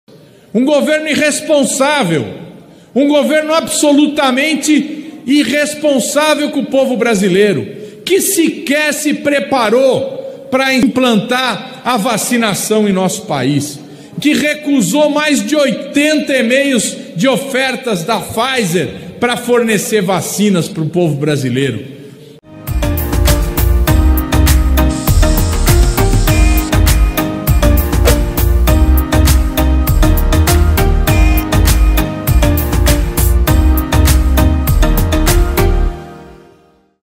forabolsonaro Governo Bolsonaro é irresponsável e o resultado é o caos!
Governo Bolsonaro é irresponsável! Não se preparou para vacinar a população brasileira e ainda recusou 80 ofertas de vacina! O resultado é o caos! Imunização a passos lentos e milhares de mortes!